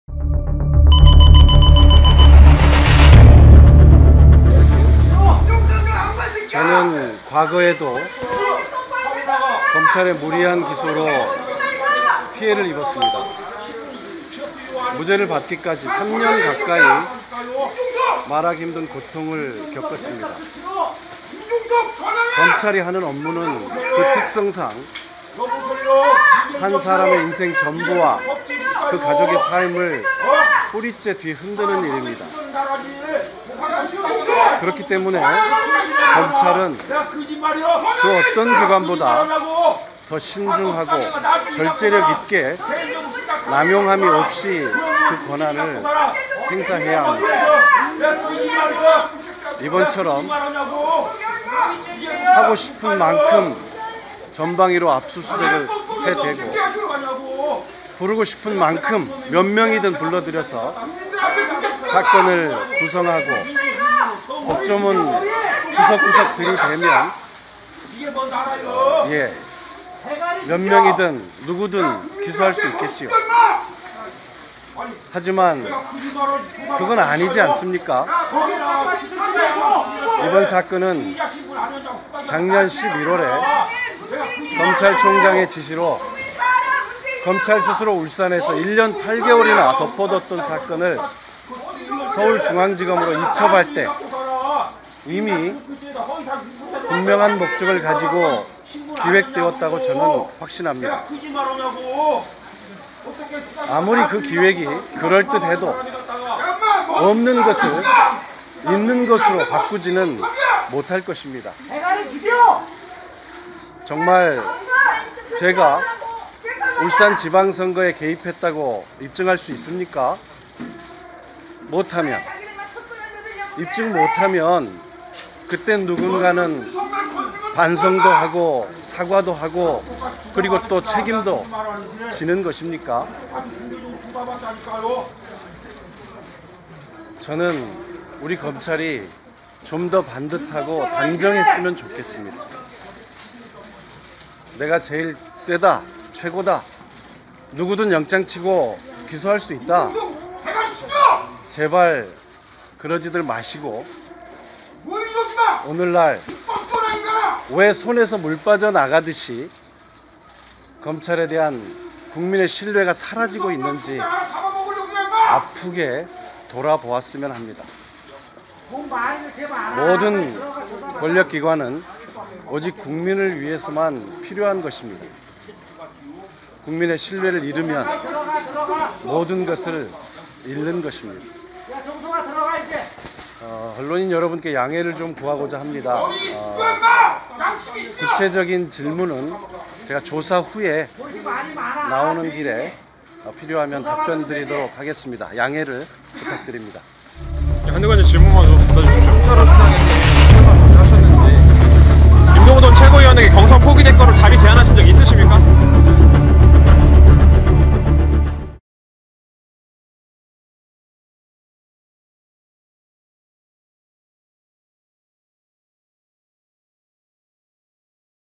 임종석, “피의자신분” 검찰출석 (서울1TV)
임종석, 검찰 출석.. 무리한 기획수사 비판

임종석 전 비서실장 30일 피의자신분 출석 임종석 '적극적인 해명'

'선거개입 의혹' 임종석 전 청와대 비서실장 검찰 출석

임종석, 검찰 출석 "이번 수사, 분명한 목적으로 기획됐다."

임 전 실장은 또 "검찰이 좀 더 반듯하고 단정했으면 좋겠다.

진중권 "임종석 언론플레이로 여론을 조작하고 지지자들 선동하지 마라